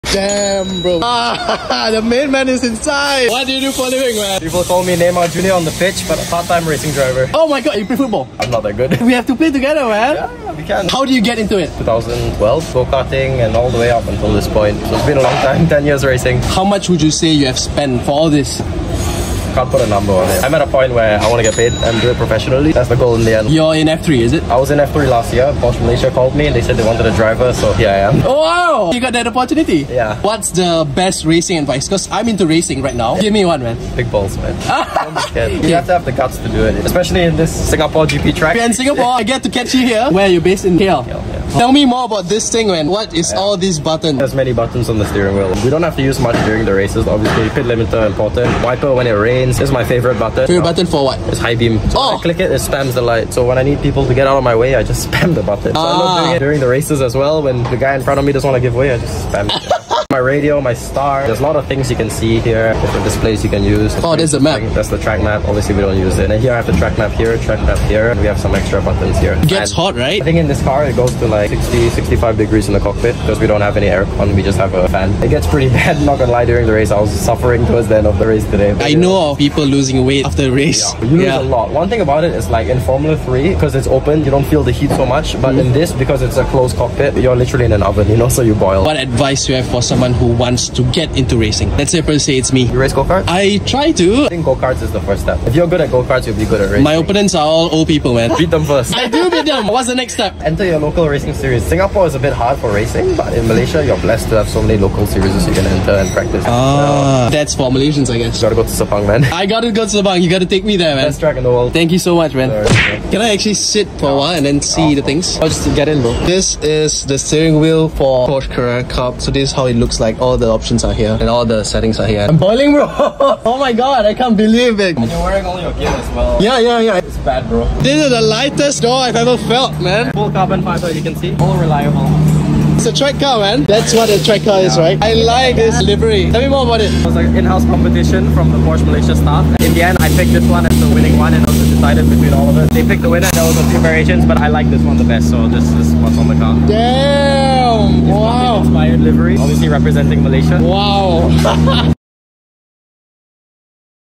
ADRENALINE CHARGED World of Racing!!!🏎️🏁
Inside the cockpit: A firsthand look into the adrenaline-charged world of racing as we interview @nazim_azman, exploring the intricate details of their high-speed domain from behind the wheel!